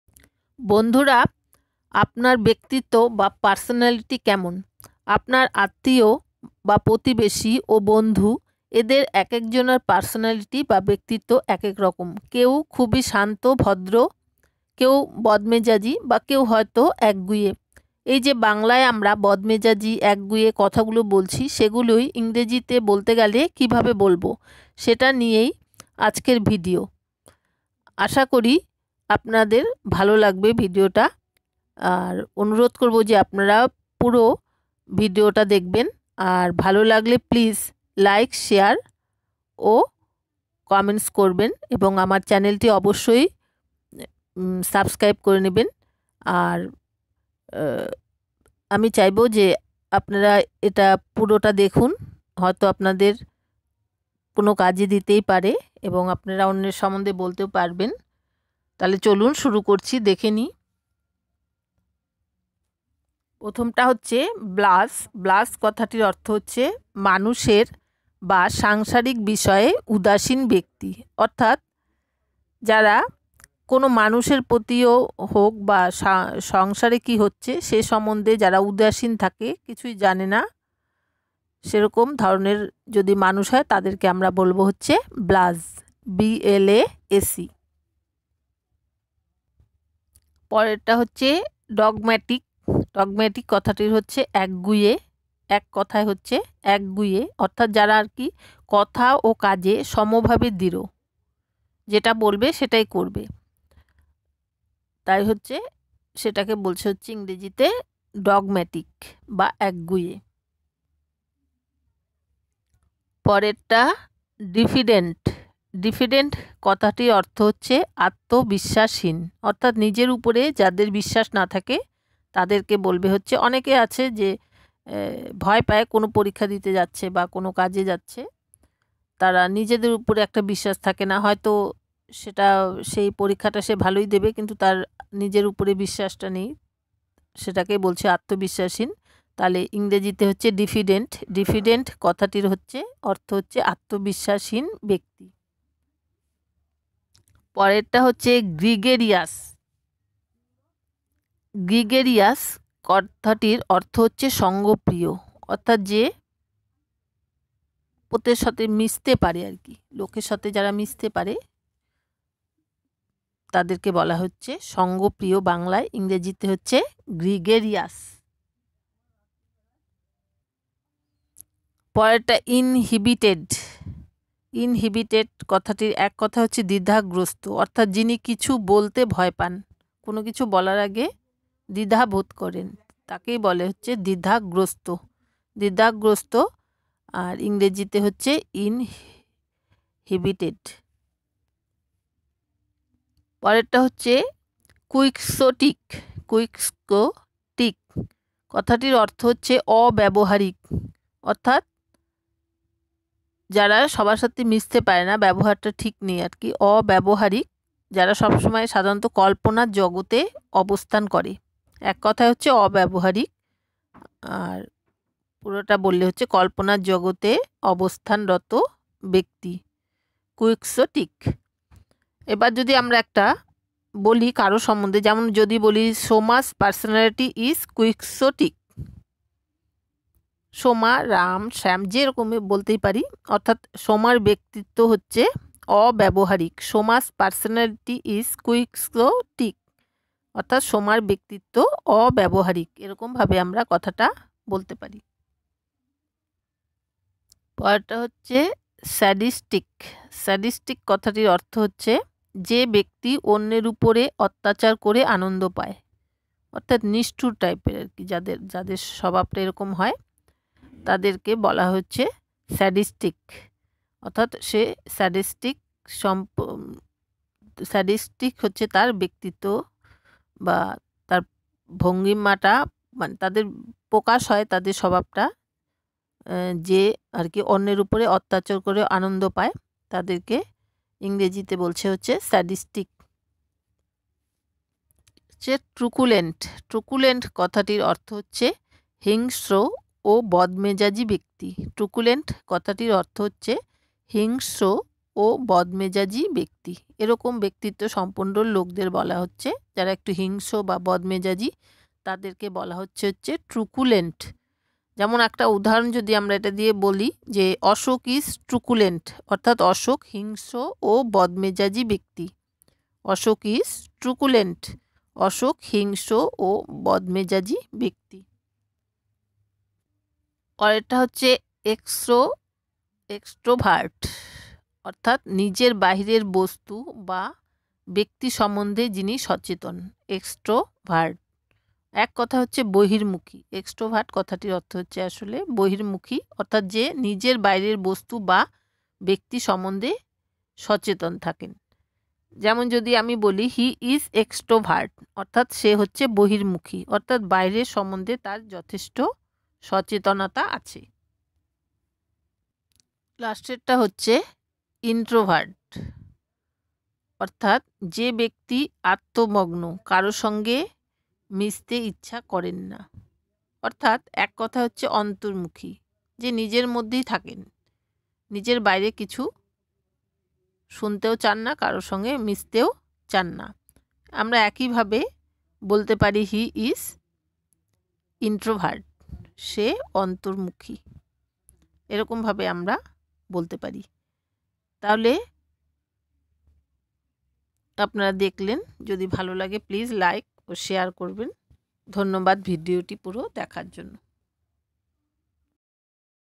আপনার ব্যক্তিত্ব কেমন অর্থাৎ আপনি কোমল মনের মানুষ না সঙ্গপ্রিয় তা কিভাবে ইংরেজিতে প্রকাশ করবেন।
আপনার ব্যক্তিত্ব কেমন?
আপনি একগুঁয়ে বা আপনি যার সাথে কথা বলছেন,
 সে বদমেজাজী। সেটা ইংরেজিতে কিভাবে বলবেন।আপনার ব্যক্তিত্ব কেমন অর্থাৎ আপনি কোমল মনের মানুষ না সঙ্গপ্রিয় তা কিভাবে ইংরেজিতে প্রকাশ করবেন।